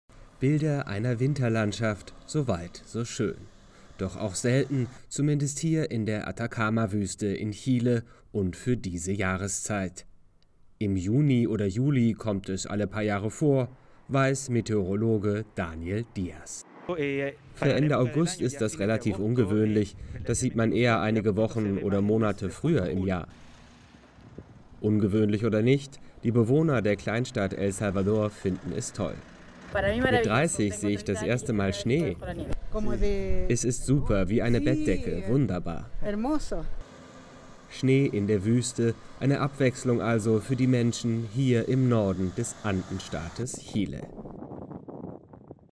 Schnee in der chilenischen Atacama-Wüste
In Chile ist dieser Tag an einem ungewöhnlichen Ort Schnee gefallen. So mancher Erwachsener in der Atacama-Wüste sieht eine Winterlandschaft zum ersten Mal im Leben.